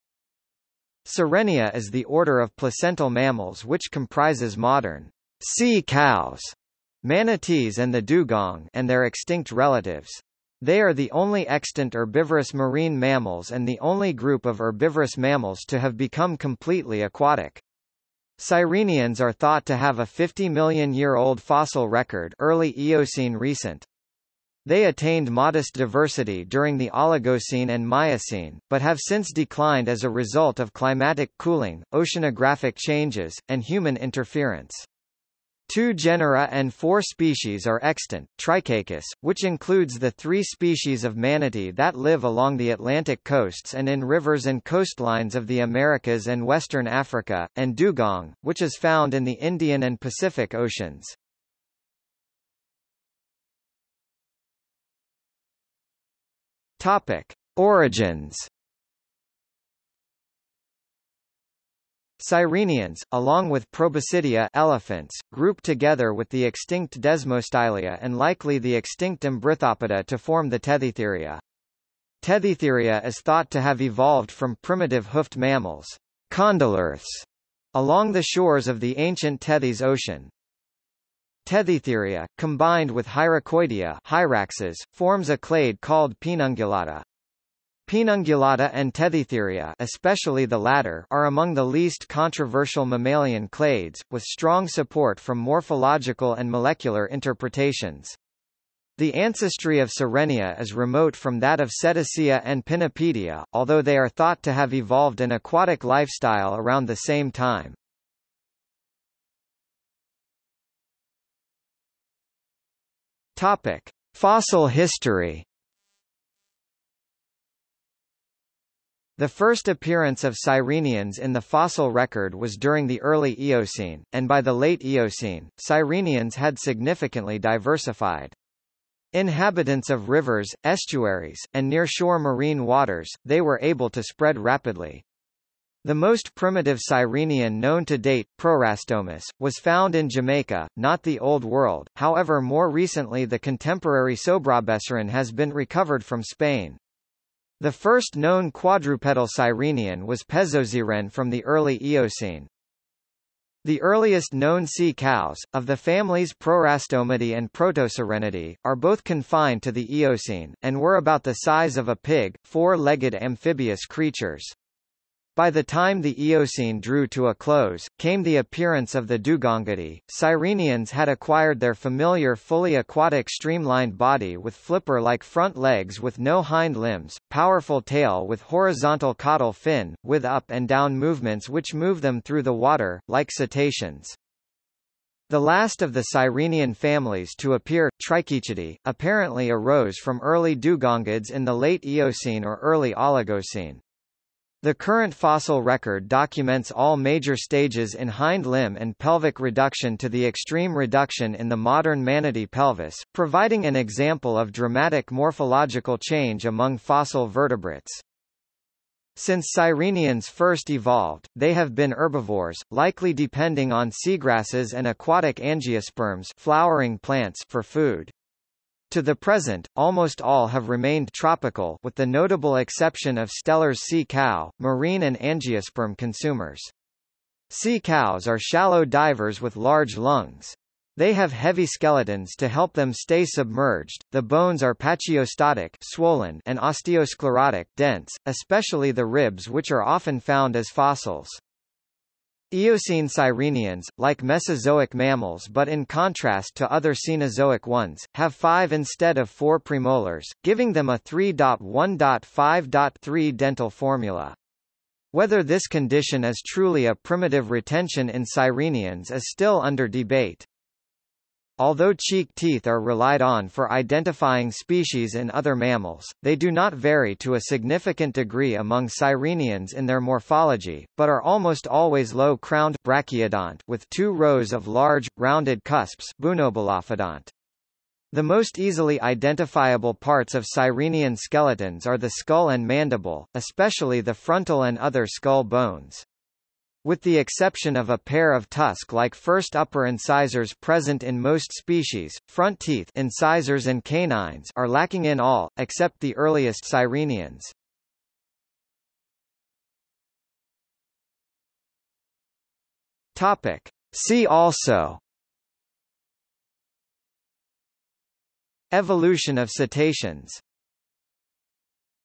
Evolution of sirenians | Wikipedia audio article
00:00:53 1 Origins
00:01:49 2 Fossil history
00:04:59 3 See also

- Socrates

SUMMARY
Sirenia is the order of placental mammals which comprises modern "sea cows" (manatees and the Dugong) and their extinct relatives. They are the only extant herbivorous marine mammals and the only group of herbivorous mammals to have become completely aquatic. Sirenians are thought to have a 50-million-year-old fossil record (early Eocene-recent). They attained modest diversity during the Oligocene and Miocene, but have since declined as a result of climatic cooling, oceanographic changes, and human interference. Two genera and four species are extant: Trichechus, which includes the three species of manatee that live along the Atlantic coasts and in rivers and coastlines of the Americas and western Africa, and Dugong, which is found in the Indian and Pacific oceans.